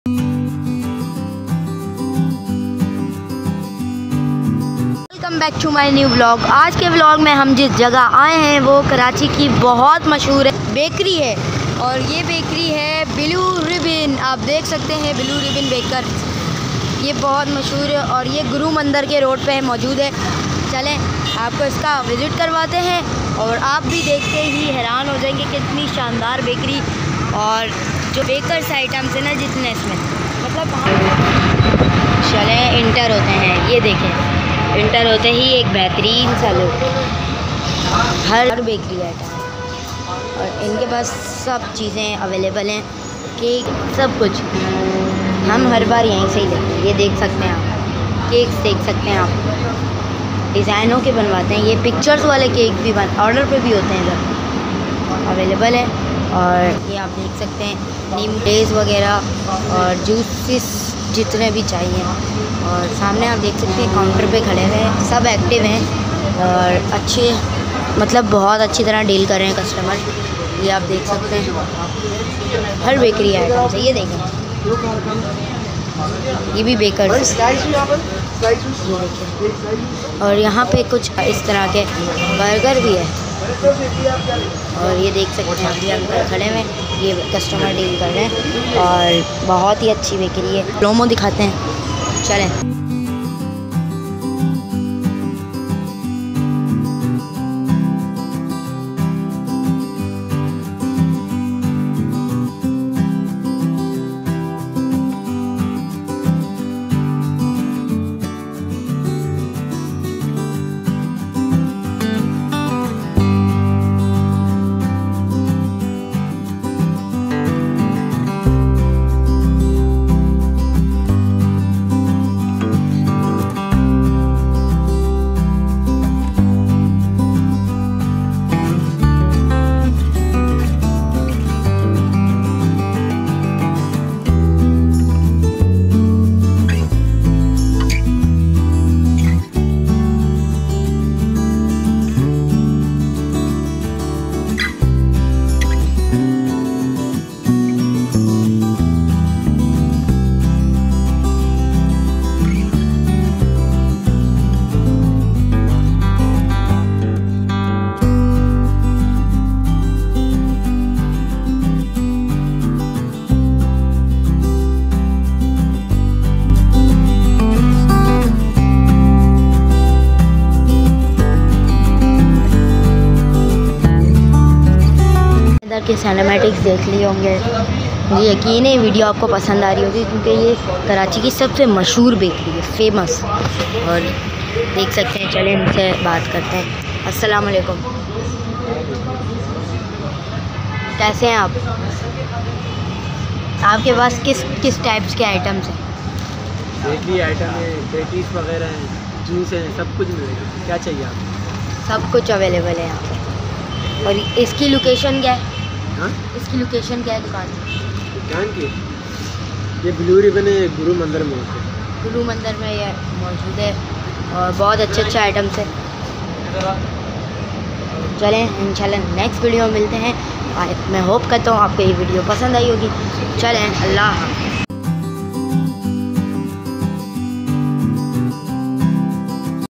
Famous Bakery in Karachi Blue Ribbon Bakers | Gurumandir Karachi | ZFA Vlogs
Blue Ribbon Bakers famous bakery in Gurumandir Karachi, they have huge setup with a wide variety of sweets, nimco, cakes, bakery item and confectioneries. it was amazing experience everything was so fresh plus their staff members were so sweet. wish them best of luck.

Topic:
Famous Bakery in Karachi Blue Ribbon Bakers | Gurumandir Karachi